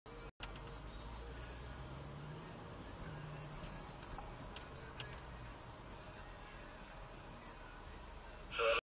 hradečák a semafor
jízda na červenou...aneb pražský sport. Ale nejel rychle, takže je to vlastně v pořádku, protože rychlost zabíjí!